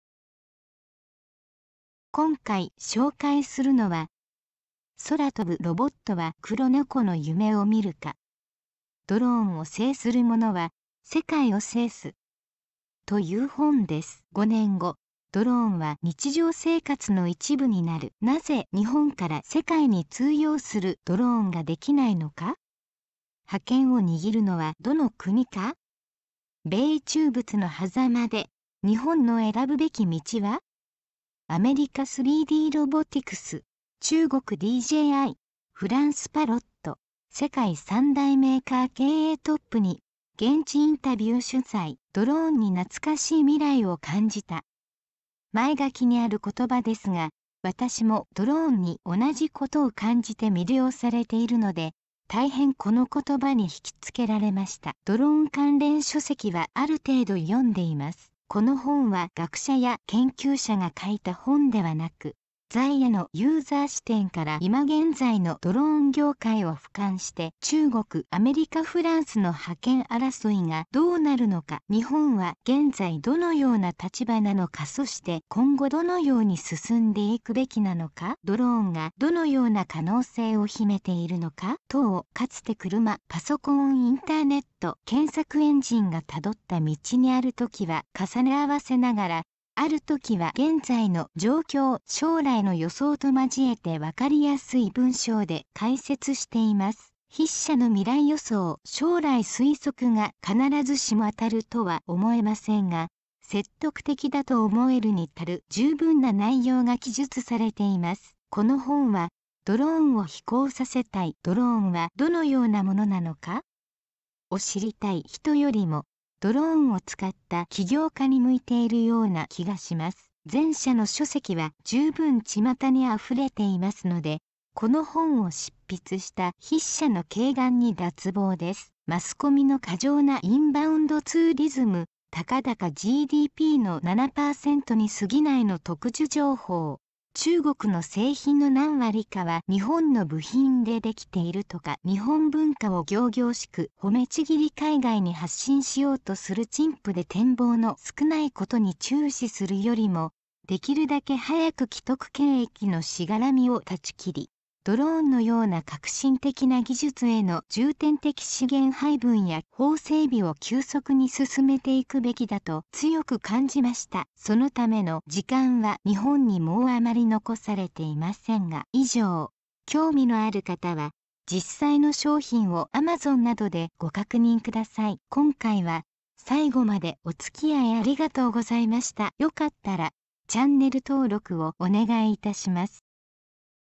【紹介】空飛ぶロボットは黒猫の夢を見るか ドローンを制する者は、世界を制す （高城 剛）
今回紹介するのは、　空飛ぶロボットは黒猫の夢を見るか ドローンを制する者は、世界を制す　という本です。

5年後、ドローンは日常生活の一部になる。なぜ日本から世界に通用するドローンができないのか?覇権を握るのはどの国か?米中仏の狭間で、日本の選ぶべき道は?アメリカ3Dロボティクス、中国DJI、フランスパロット、世界3大メーカー経営トップに、現地インタビュー取材!

「ドローン」に懐かしい未来を感じた！前書きにある言葉ですが、私もドローンに同じことを感じて魅了されているので、大変この言葉に引き付けられました。ドローン関連書籍はある程度読んでいます。この本は学者や研究者が書いた本ではなく、在野のユーザー視点から今現在のドローン業界を俯瞰して、
中国・アメリカ・フランスの覇権争いがどうなるのか？
日本は現在どのような立場なのかそして今後どのように進んでいくべきなのか？
ドローンがどのような可能性を秘めているのか？
等をかつて車・パソコン・インターネット・検索エンジンが辿った道にある時は重ね合わせながら、ある時は現在の状況・将来の予想と交えてわかりやすい文章で解説しています。筆者の未来予想・将来推測が必ずしも当たるとは思えませんが、説得的だと思えるに足る十分な内容が記述されています。
この本は、ドローンを飛行させたい・ドローンはどのような物なのか？を知りたい人よりも、ドローンを使った起業家に向いているような気がします。前者の書籍は十分巷に溢れていますので、この本を執筆した筆者の慧眼に脱帽です。
マスコミの過剰なインバウンドツーリズム（たかだかGDPの7%に過ぎない）の特需情報、中国の製品の何割かは日本の部品でできているとか日本文化を仰々しく褒めちぎり海外に発信しようとする陳腐で展望の少ない事に注視するよりも、できるだけ早く既得権益のしがらみを断ち切り、ドローンのような革新的な技術への重点的資源配分や法整備を急速に進めていくべきだと強く感じました。そのための時間は日本にもうあまり残されていませんが。